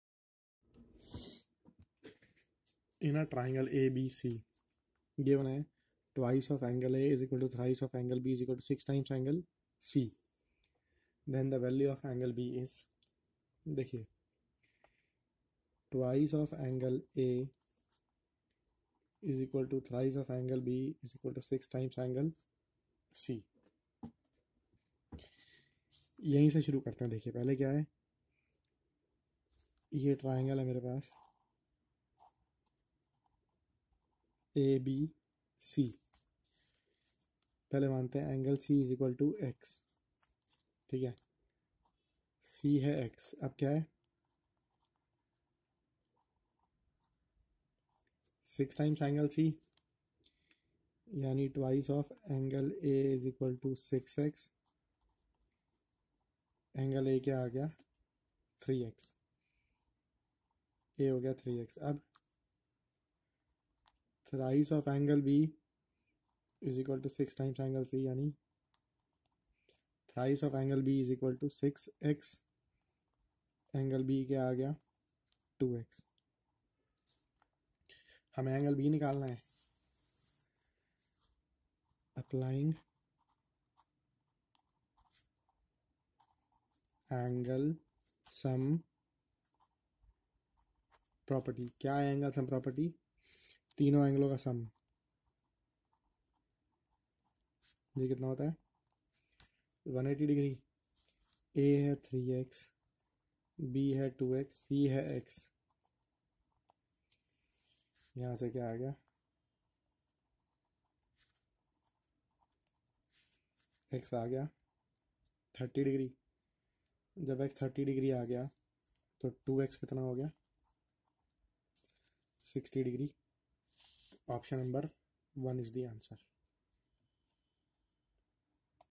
tsp 12
12. In a 6 ABC, if 2 ∠A = 3 ∠B = 6 ∠C, value of ∠B is

(SSC CHSL DEO & LDC Exam. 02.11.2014 (IInd Sitting)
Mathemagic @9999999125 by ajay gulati